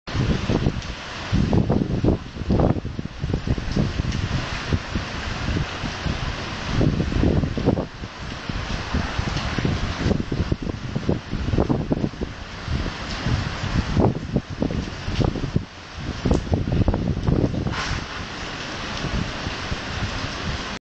Bay's A Risen: Monday October 29, 2012 (3-2)
The rising bay from Hurricane Sandy in Ocean City, Maryland.

If you look very, very carefully, you can see that a boat has sunk. All you can see is the top of the awning area. It's on the far side of the pier towards the right. Look for what looks like a white cap that doesn't move forward. May be more visible in other videos in this series.

Hurricane Sandy Monday October 29, 2012.